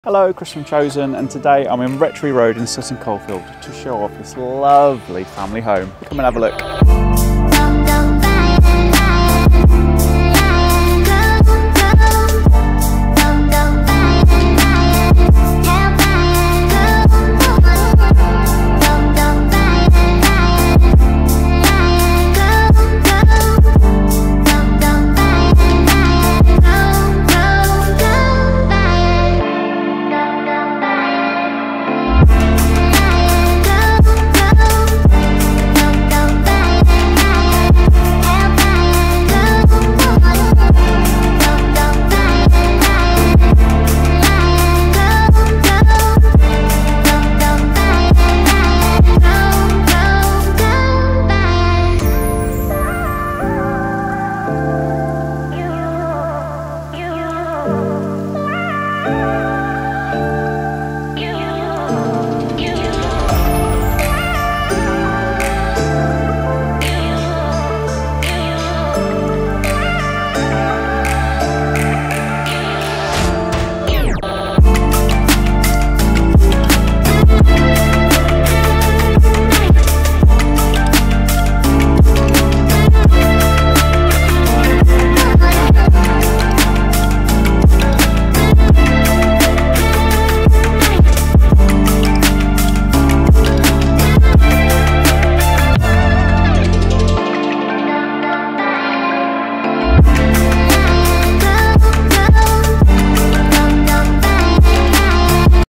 Rectory Road, Sutton Coldfield | Chosen Home | Property Tour 🏡
Why this home is Chosen...

Introducing a contemporary 4-bedroom semi-detached property, recently launched on Rectory Road in the sought-after location of Sutton Coldfield. This residence seamlessly merges style and practicality, offering a haven for modern living. Boasting excellent connectivity via public transport, a brief drive leads to Sutton Town Centre and the expansive Sutton Park, providing residents with diverse recreational and leisure options. Beyond its prime location, this property comes with the added advantage of proximity to esteemed schools, ensuring an excellent educational environment for families. With its thoughtfully designed spaces and convenient access to key amenities.

A quick tour
Highlights of this stunning family home include:

* Ideal location
* Modern and bright throughout
* Three reception rooms
* Wonderful modern kitchen
* 4 double bedrooms
* Large bathroom
* Renowned school catchment area
* Large sunny garden
* Did we mention the esteemed school catchment area?